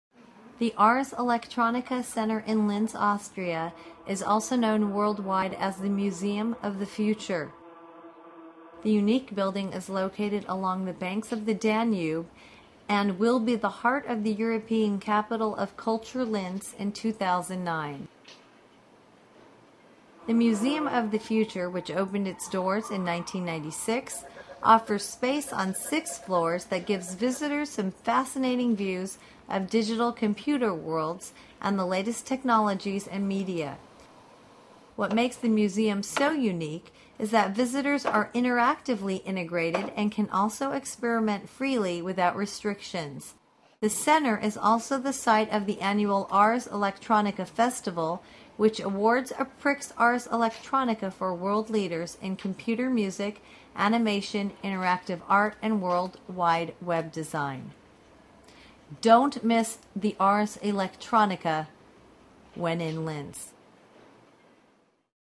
Linz Tourism: Ars Electronica Center
Linz Tourism: Ars Electronica Center. Part of the series: Linz, Austria Tourism. The Ars Electronica Center in Linz, Austria is a unique museum of the future with six floors of the latest technology and media. Visit the Ars Electronica Center, which is the site for the Ars Electronica Festival, in Lina with travel tips in this free video on tourism.